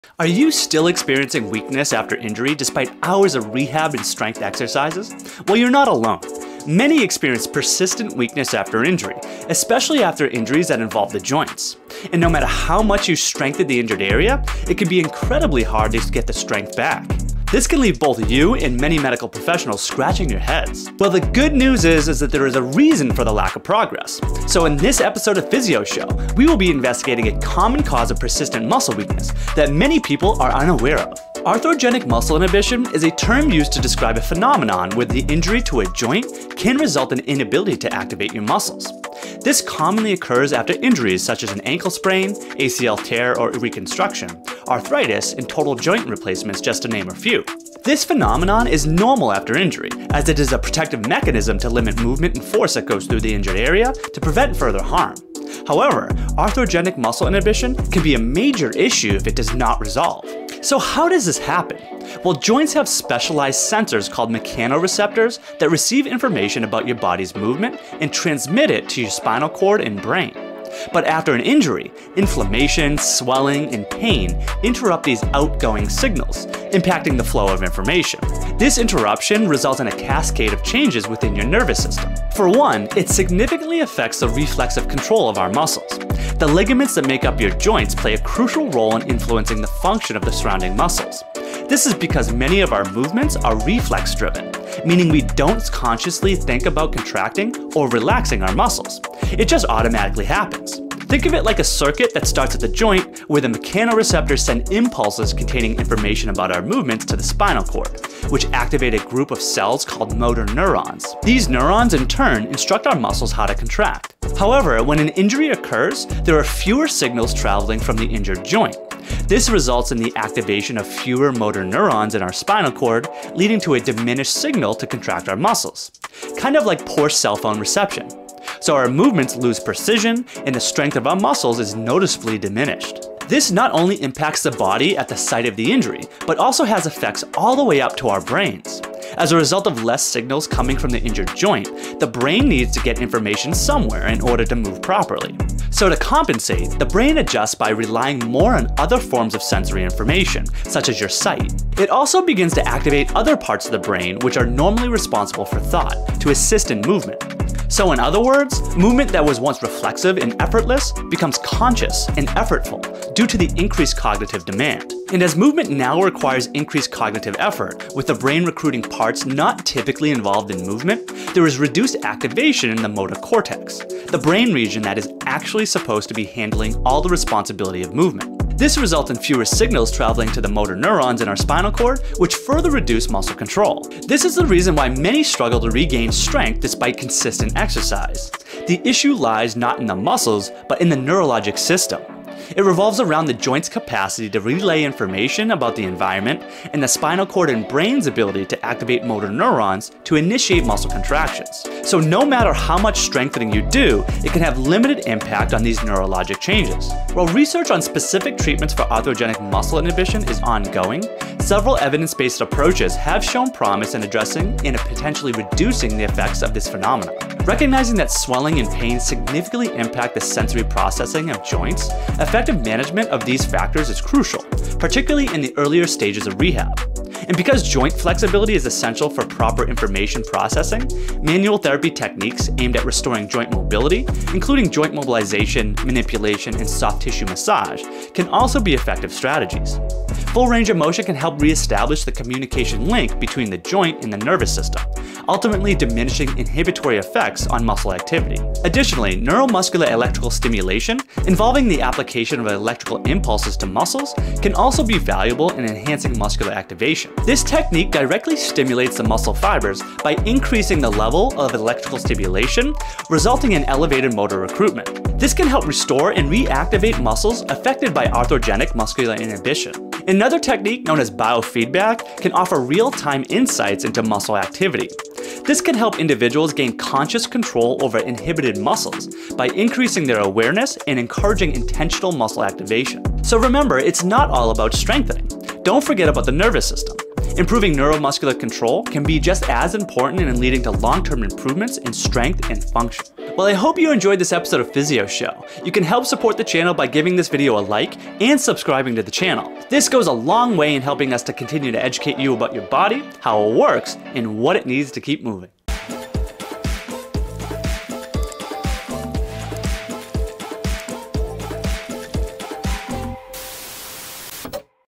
Chronic Muscle Weakness- Why You're Not Getting Stronger After Injury or Surgery
Struggling with lingering weakness after injury or surgery? You're not alone! Join us on PhysioShow as we unravel the mysteries behind arthrogenic muscle inhibition, a common cause of long-term muscle weakness, providing insights that both you and medical professionals might have missed!

Host: Dr. Bobby Dattilo, PT, DPT
Board Orthopedic Clinical Specialist